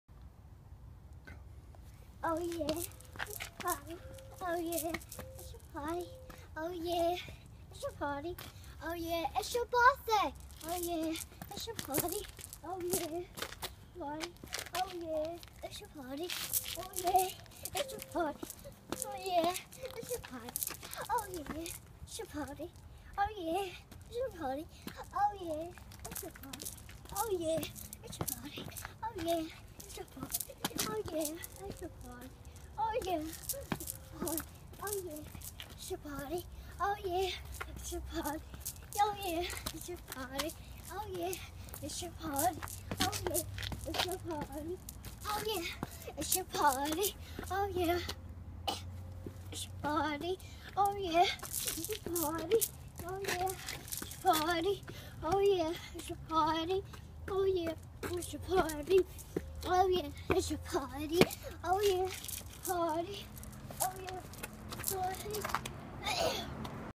Skye, Laila, Corey and Zuri
Laila dancing in the driveway march 19 2015